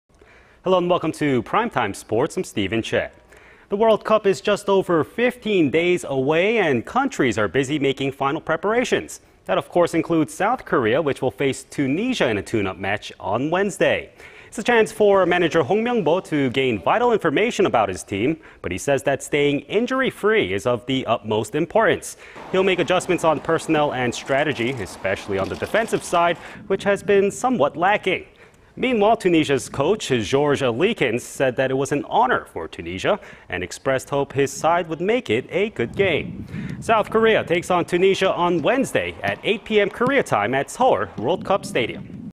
Football: S. Korea to take on Tunisia in friendly
Hello, and welcome to Prime Time Sports. I'm Steven Choi .
The World Cup is just over 15 days away and countries are busy making final preparations. That, of course, includes South Korea, which will face Tunisia in a tune-up match on Wednesday.
It's a chance for Manager Hong Myung-bo to gain vital information about his team,... but he says that staying injury-free is of the utmost importance.

He'll make adjustments on personnel and strategy,... especially on the defensive side,... which has been somewhat lacking.

Meanwhile, Tunisia's coach Georges Leekens said it was an honor for Tunisia... and expressed hope his side would make it a good game.

South Korea takes on Tunisia on Wednesday at 8 p.m., Korea time,... at Seoul World Cup Stadium.